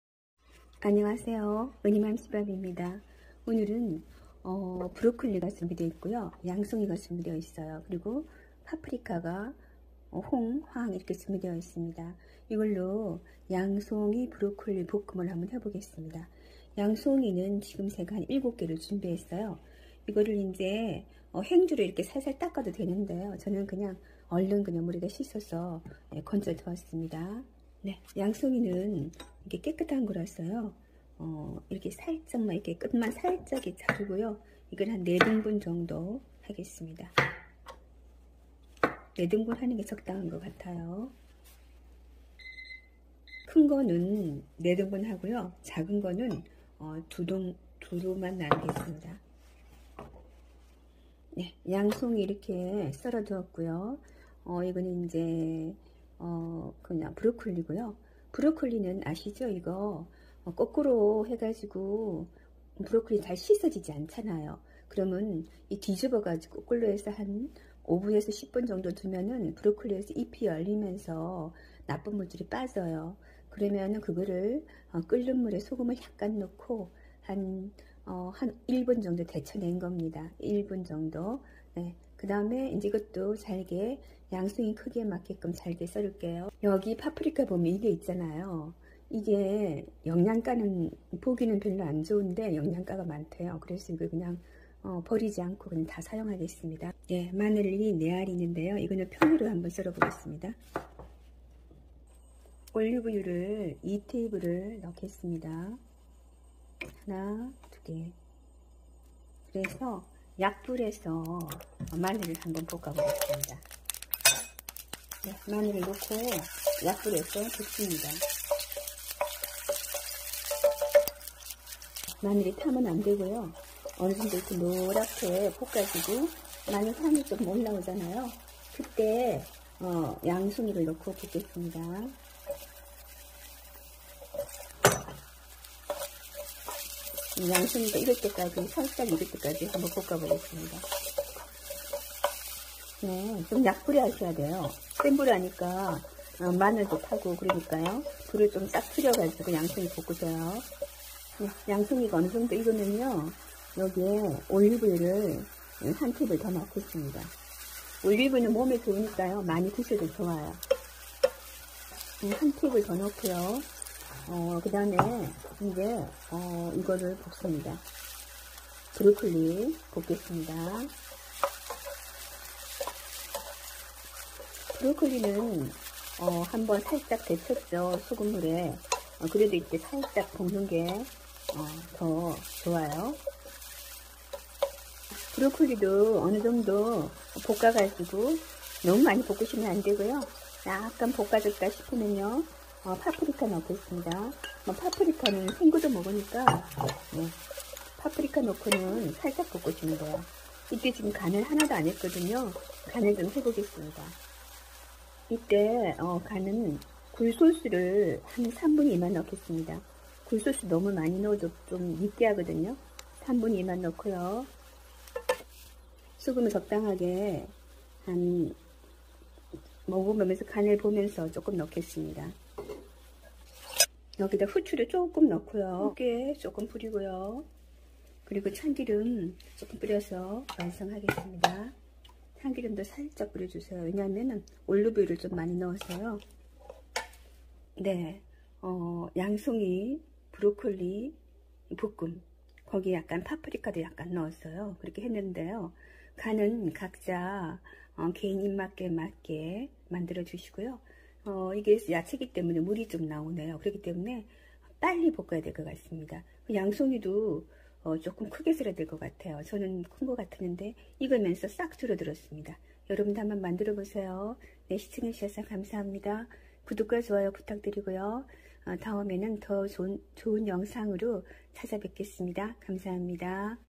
양송이 브로콜리 볶음# 색깔 만큼 맛있는 건강반찬이예요~
*양송이  큰거 7개 (많아도됌)

*브로콜리 반개

*홍ㆍ황파프리카  반개씩

*통마늘  적당히

* 올리브유  3T

*굴소스

*소금  통깨 참기름

   감사합니다~